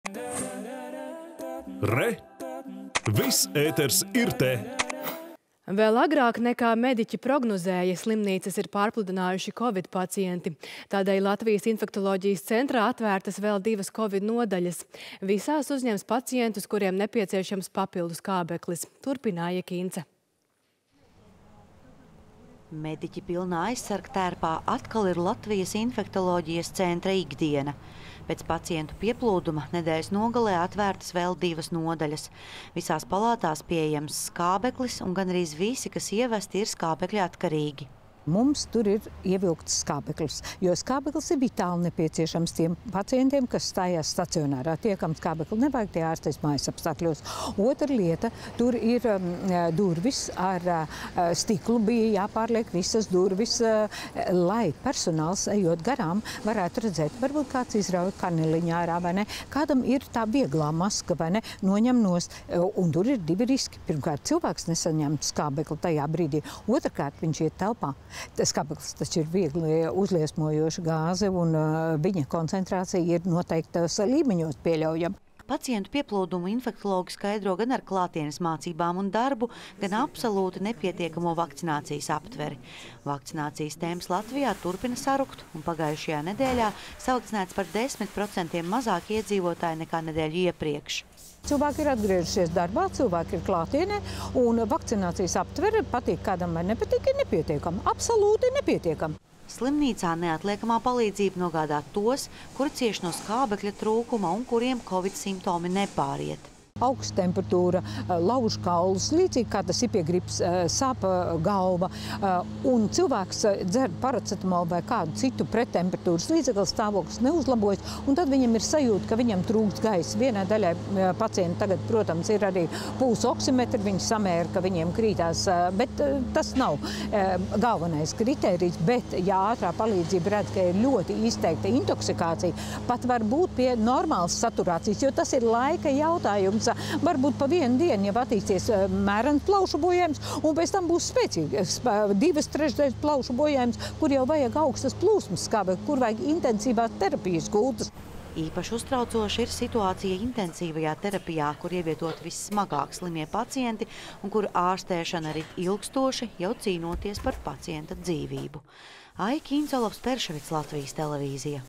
LIC atver divas jaunas nodaļas Covid pacientiem
Vēl agrāk, nekā mediķi prognozēja, slimnīcas ir  pārplūdinājuši kovid pacienti. Tādēļ Latvijas Infektoloģijas centrā šodien atvērtas vēl divas kovid nodaļas. Visās tiks uzņemti pacienti, kuriem nepieciešams papildu skābeklis. Turpina Aija Kinca.